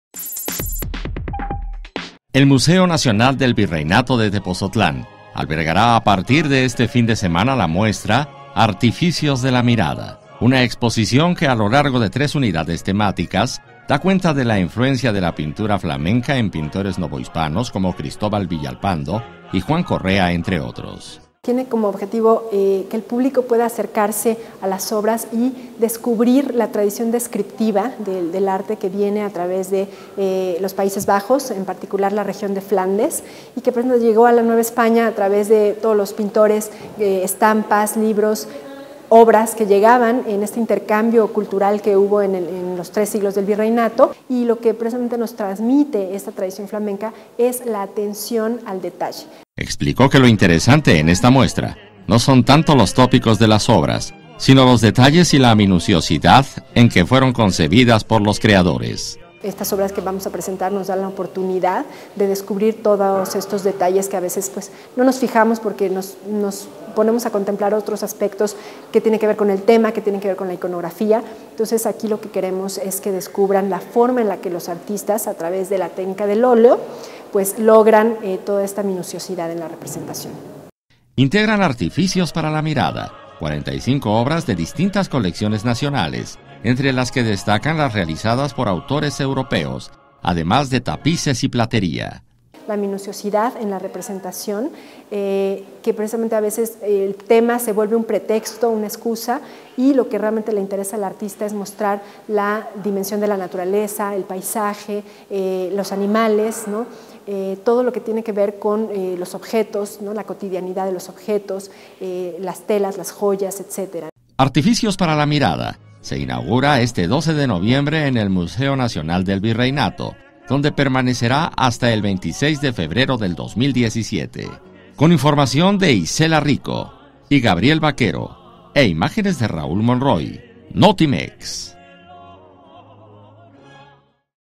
"Artificios para la mirada", la influencia flamenca en pintores novohispanos
Obras de creadores europeos en diálogo con artistas de la Nueva España que se inaugura en el Museo Nacional del Virreinato